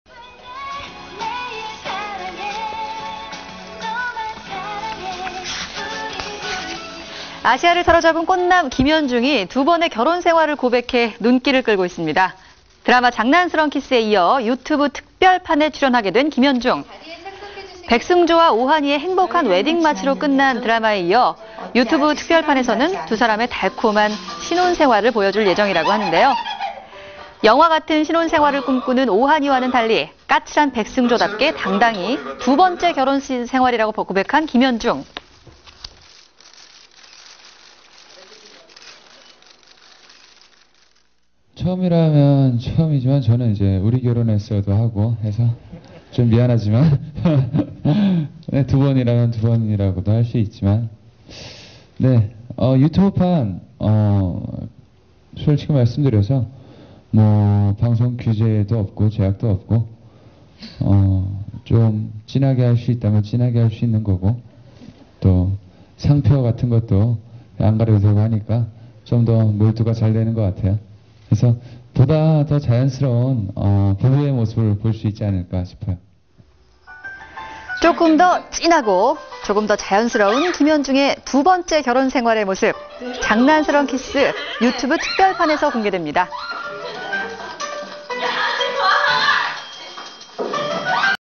[news] kim hyun jung,'Janki'Special Editions ( 김현중, "'장키' 특별판 더 진하게)
아시아를 사로잡은 꽃남! 김현중이 두 번의 결혼생활을 고백해 눈길을 끌고 있습니다.
드라마 '장난스런 키스'에 이어 유튜브 특별판에 출연하게 된 김현중!
'백승조'와 '오하니'의 행복한 웨딩마치로 끝이 난 드라마에 이어 유튜브 특별판에서는 두 사람의 달콤~한 신혼생활을 보여줄 예정이라고 하는데요.
영화 같은 신혼생활을 꿈꾸는 '오하니'와는 달리 까칠한 '백승조'답게 당당히 두 번째 결혼생활이라고 고백한 김현중!

[인터뷰:김현중]
Q) 드라마 속 결혼생활에 대해?
A) 처음이라면 처음이지만 저는 '우리결혼했어요'도 하고 그래서 좀 미안 하지만 두 번이라면 두 번이라고도 할 수 있지만 유튜브판 솔직히 말씀드려서 방송규제도 없고 제약도 없고 좀 진하게 할 수 있다면 진하게 할 수

있는 거고 또 상표 같은 것도 안 가려도 되니까 좀 더 모두가 잘 되는 것 같아요. 보다 자연스러운 부부의 모습을 볼 수 있지 않을까 싶어요.

조금 더 진하고~
조금 더 자연스러운 김현중의 두 번째? 결혼생활은 모습은 '장난스런 키스' 유튜브 특별판에서 공개됩니다.